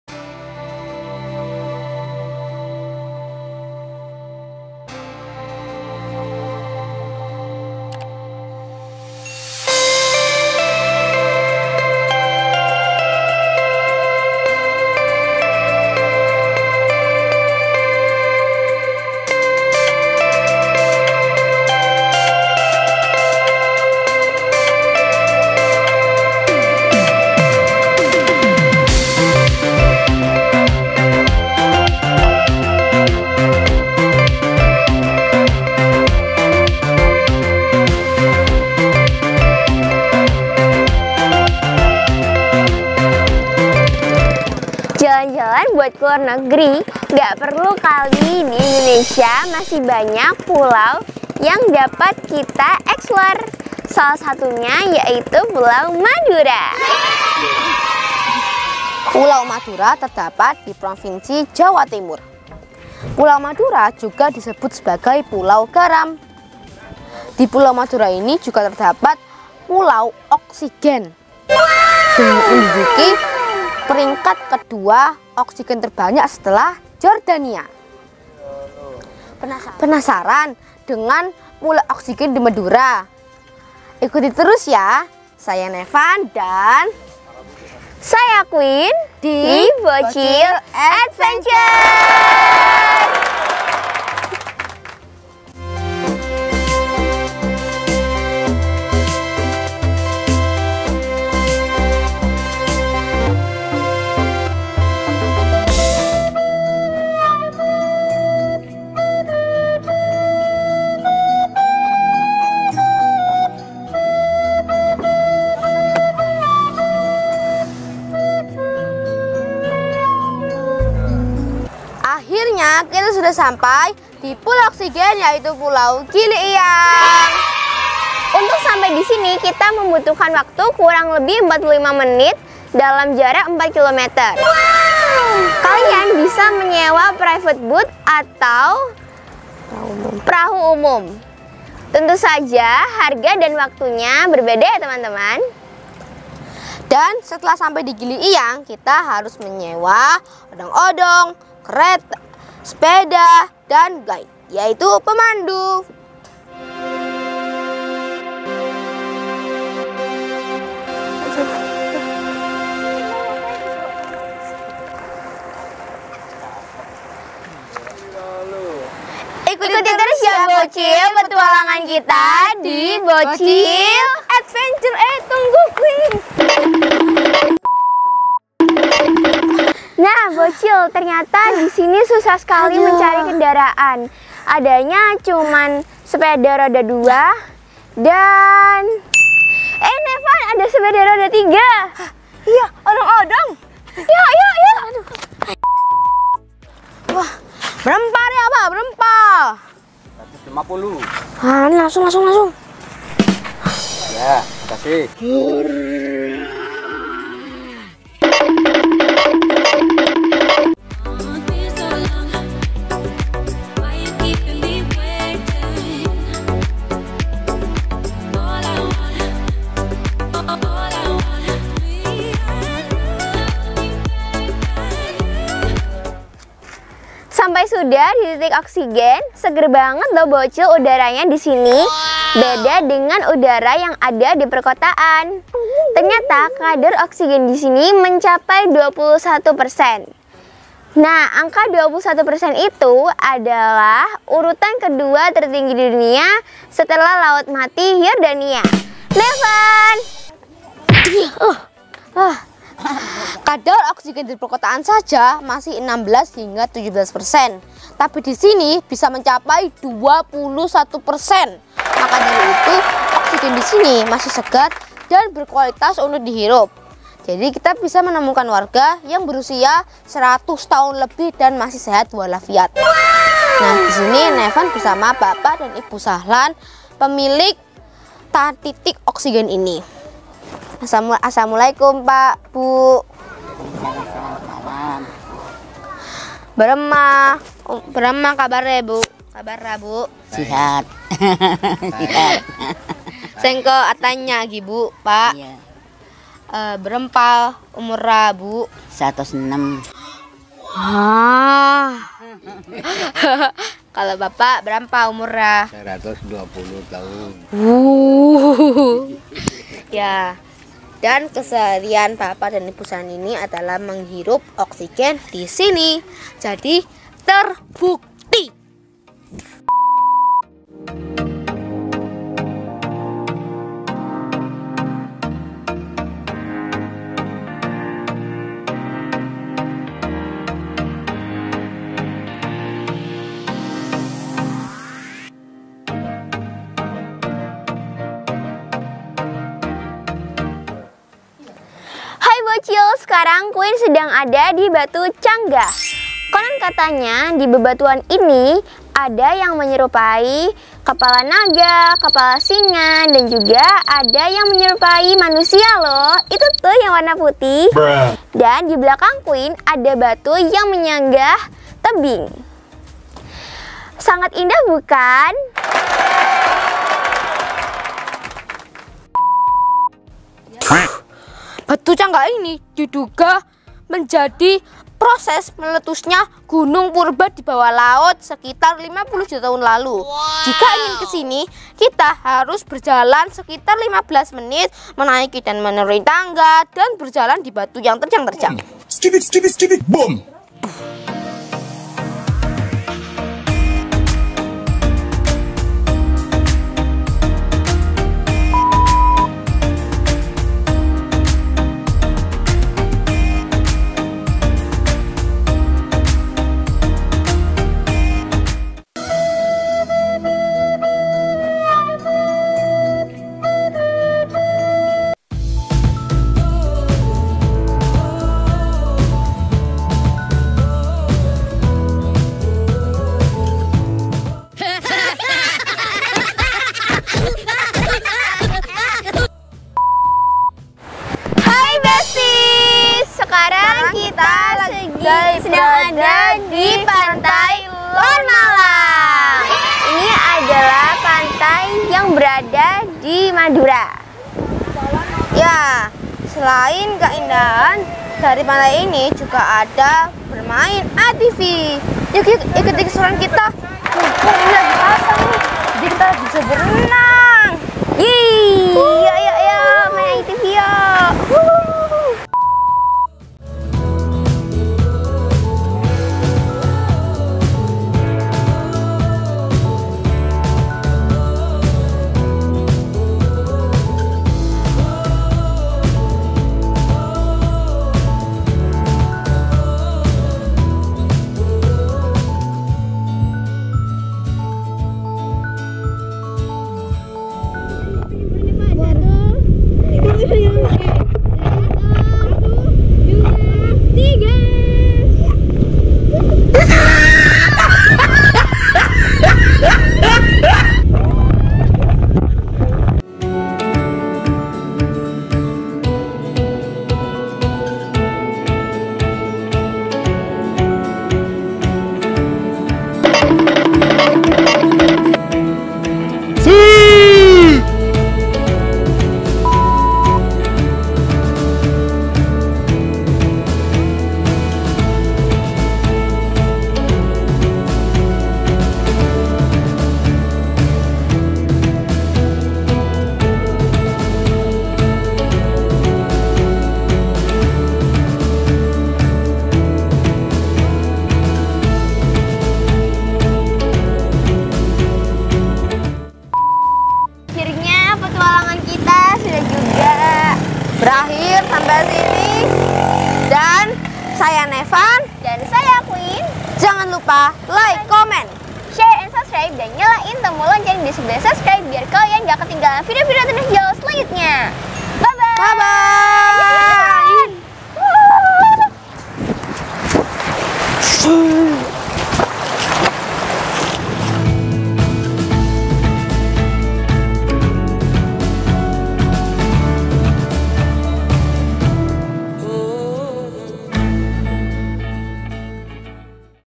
PULAU GILIYANG TITIK OKSIGEN TERBAIK KEDUA DI DUNIA DENGAN MASYARAKAT UMUR 1 ABAD - BOCIL ADVENTURE
Kandungan oksigen di pulau Giliyang mencapai 21% dan menempatkan pada urutan titik oksigen terbaik kedua di dunia. Lingkungan di pulau ini masih alami dan bersih dengan kualitas udara yang baik. Selain itu disini juga ada beberapa masyarakat yang berumur hingga 100 tahun masih aktif berkegiatan sehari-hari dan sehat bugar.

Share & Like yaaa video ini